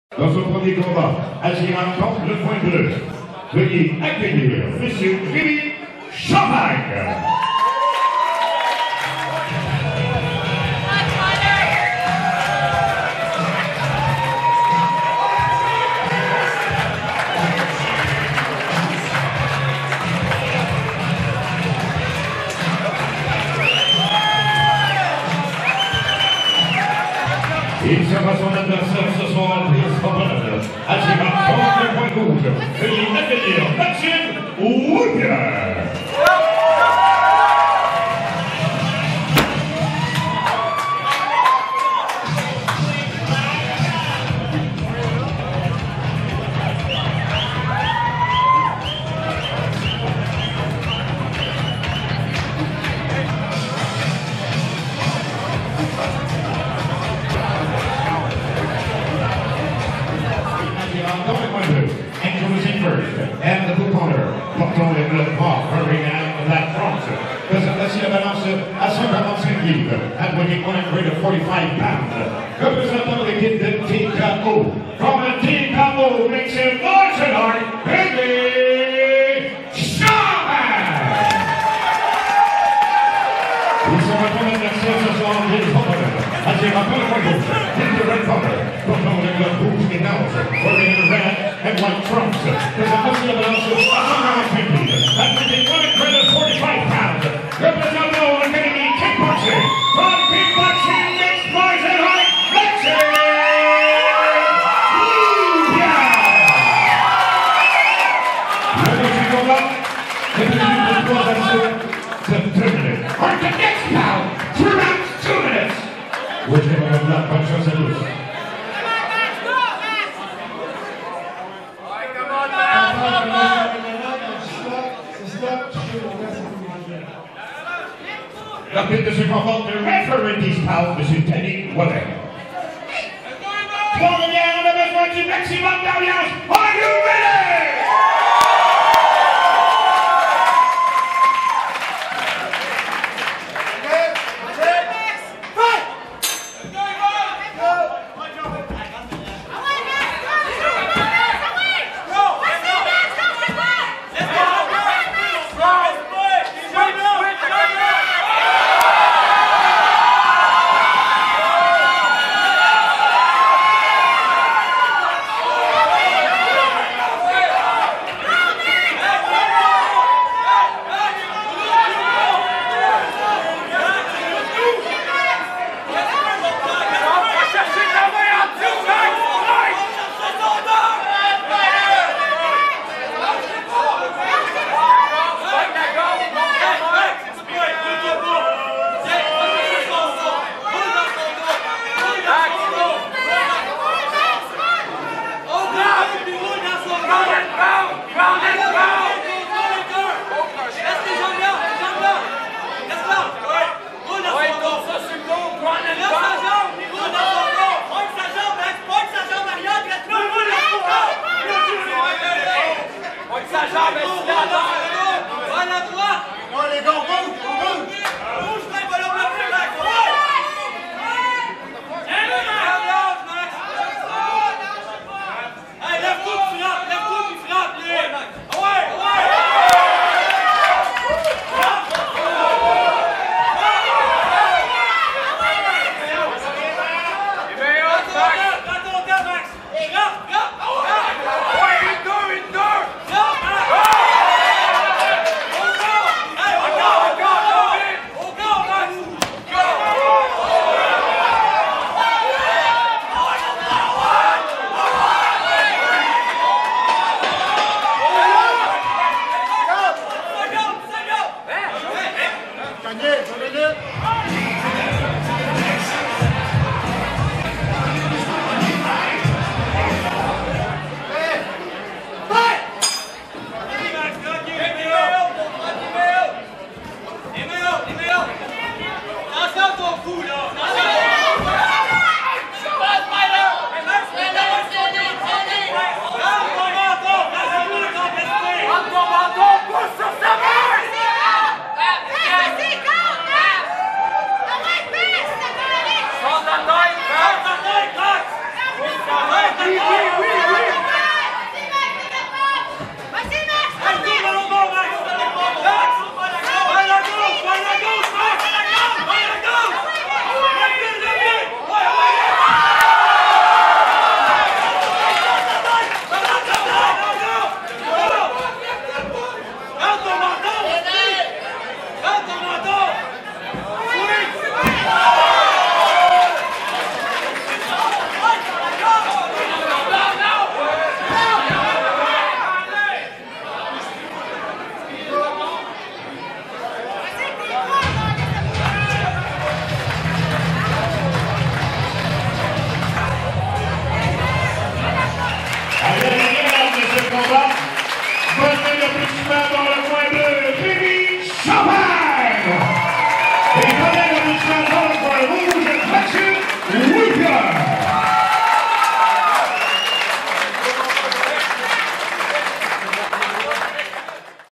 RÉMY CHAMPAGNE vs MAXIME LUPIEN
Rémy Champagne affronte Maxime Lupien lors de la soirée de combats mma amateur DFC FIGHT NIGHT 5, qui a eu lieu a l'hippodrome de Trois-Rivières, Québec, Canada le 31 mars 2018